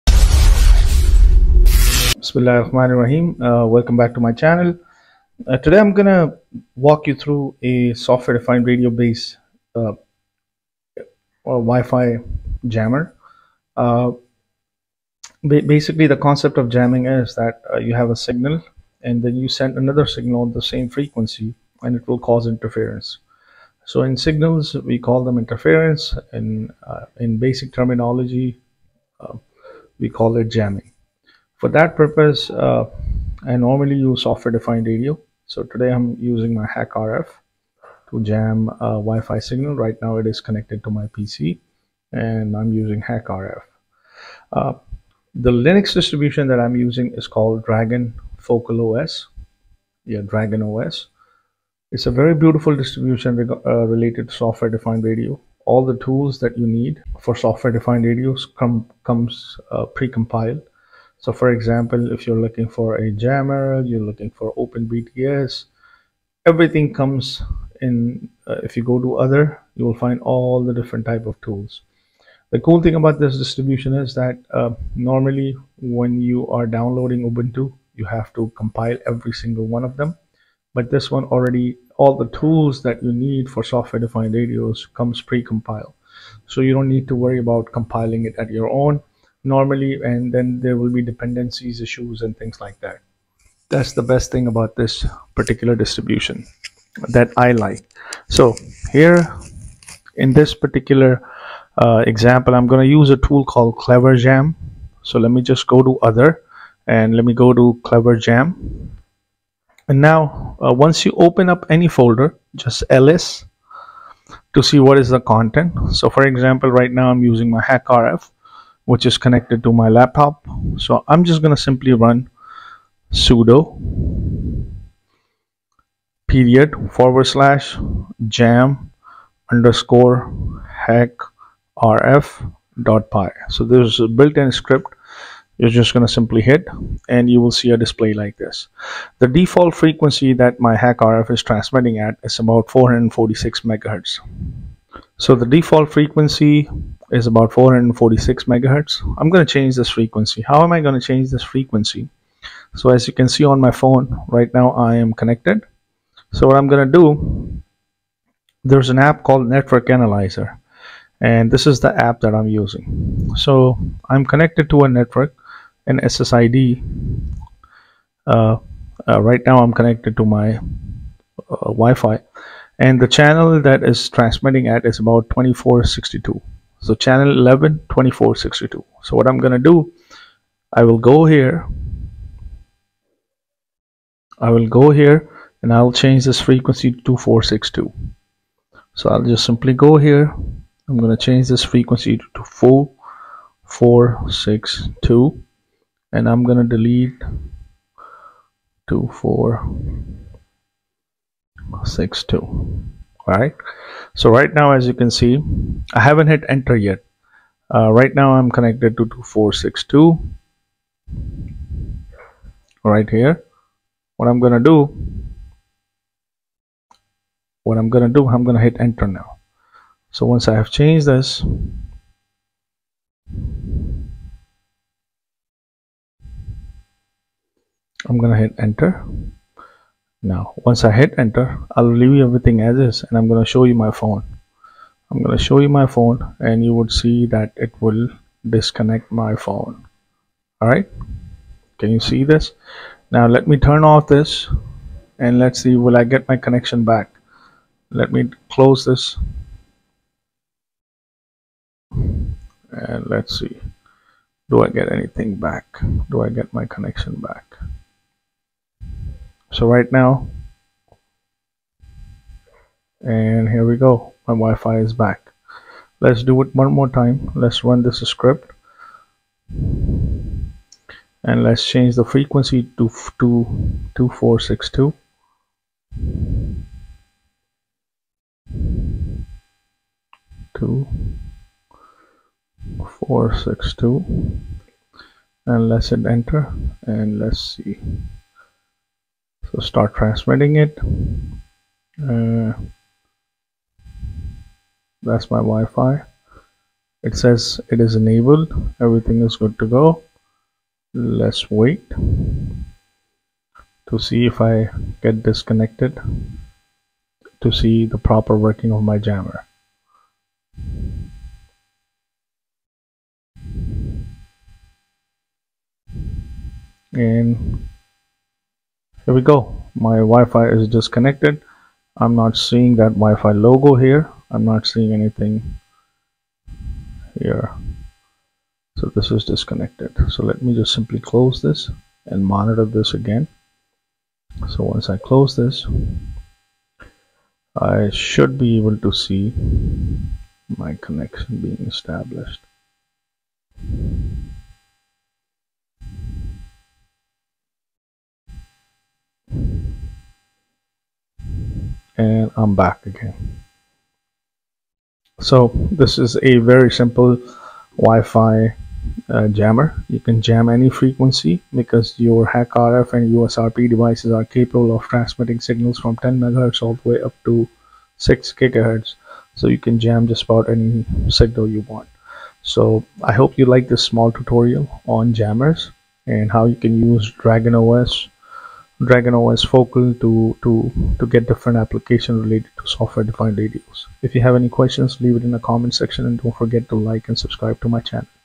How to make a Signal Jammer of any Frequency within 7 minutes | SDR 10 MHz to 6GHz
Software Defined Radio based Jammer